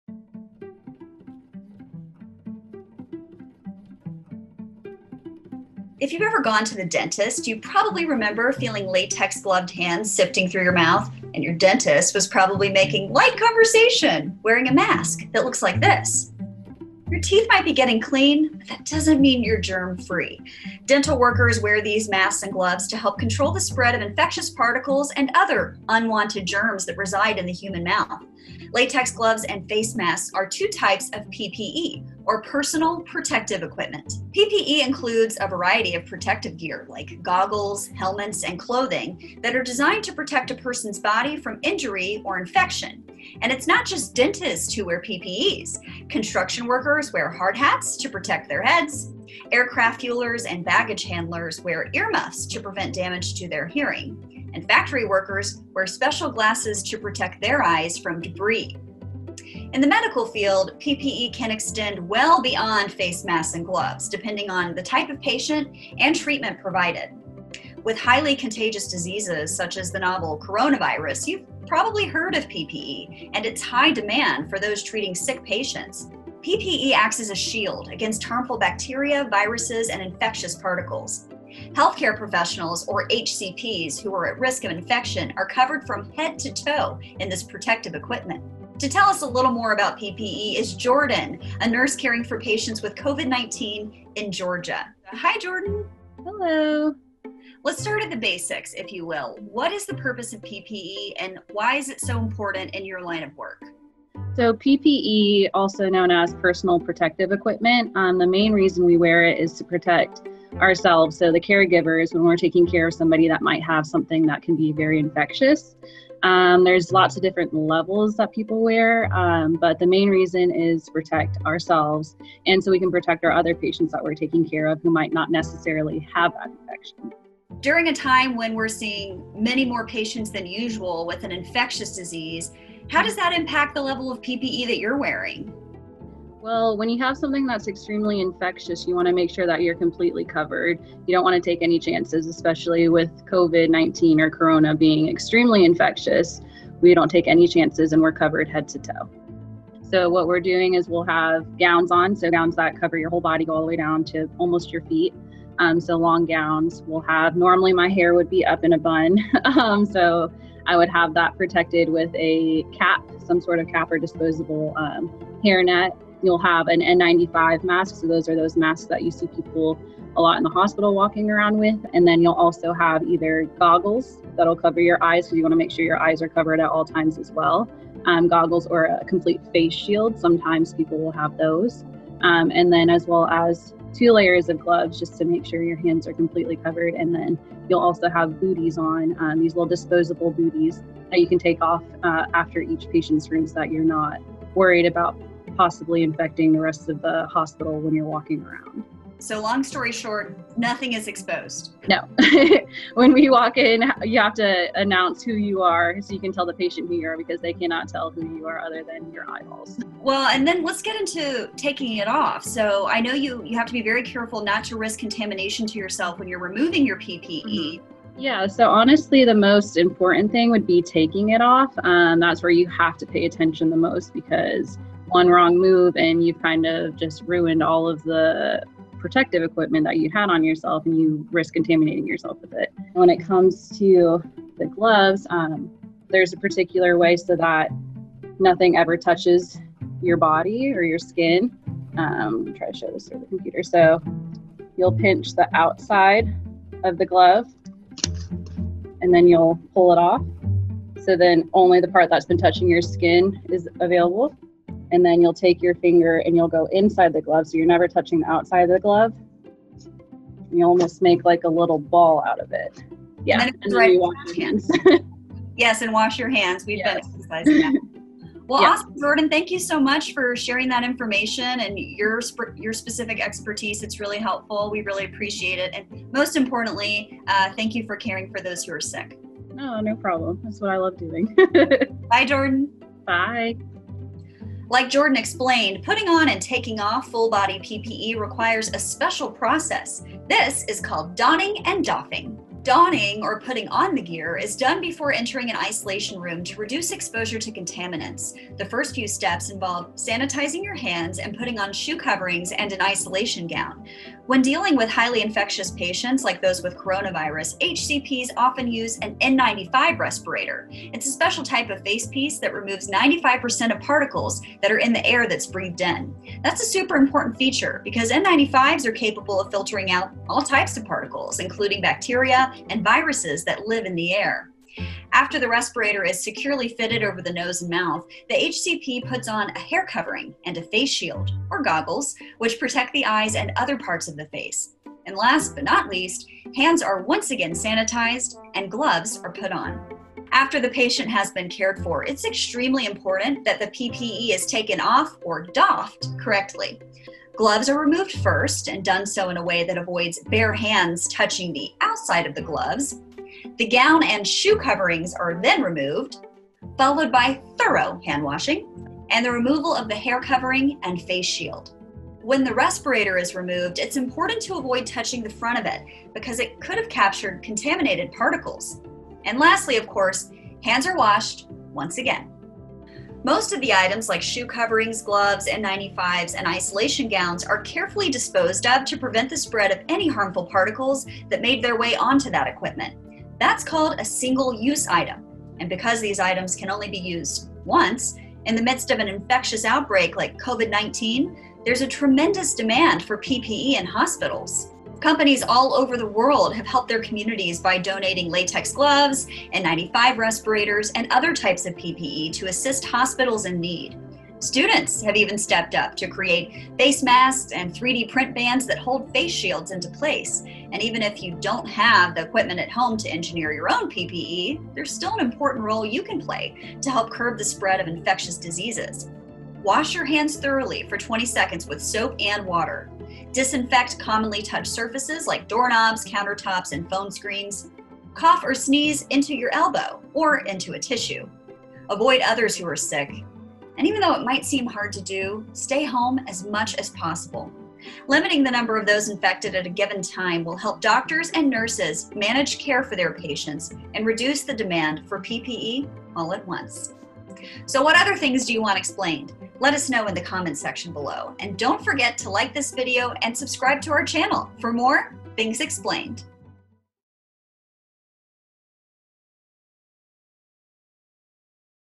Why Personal Protective Equipment (PPE) Is Important | Things Explained
Healthcare professionals wear personal protective equipments to protect themselves and patients from contaminants like bacteria and viruses. In this episode of Things Explained, we talk with a Georgia nurse who is caring for coronavirus patients and examine why PPE is in such high demand during the global pandemic.